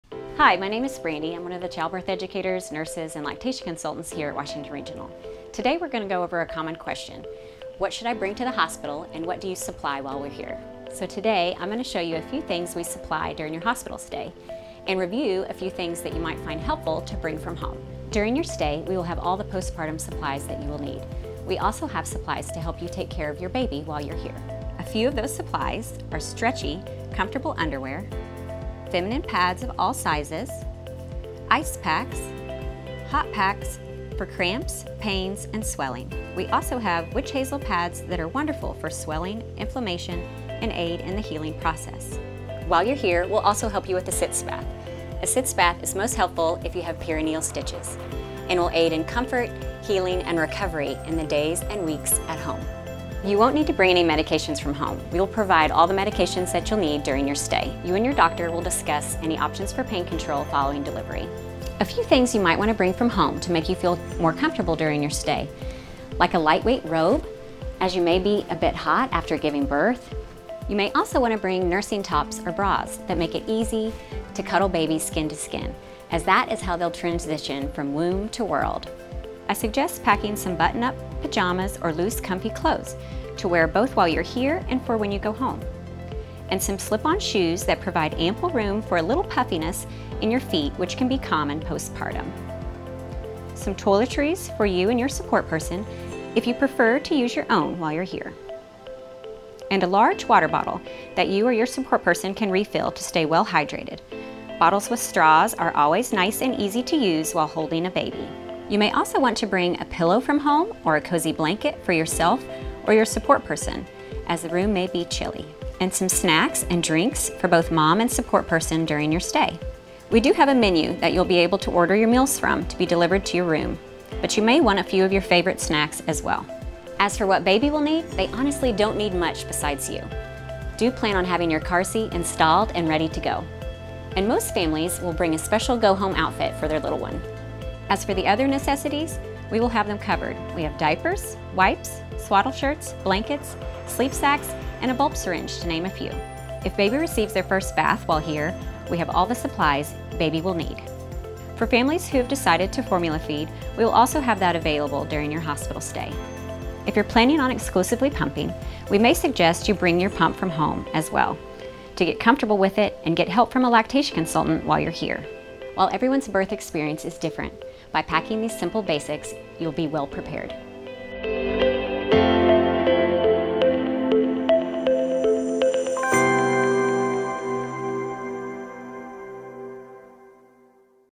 Washington Regional's Hospital Bag Checklist for Mom and Baby
Washington Regional registered nurse Brandi Sprouse talks about what moms should bring to the hospital for themselves and their babies, and what will be provided to them.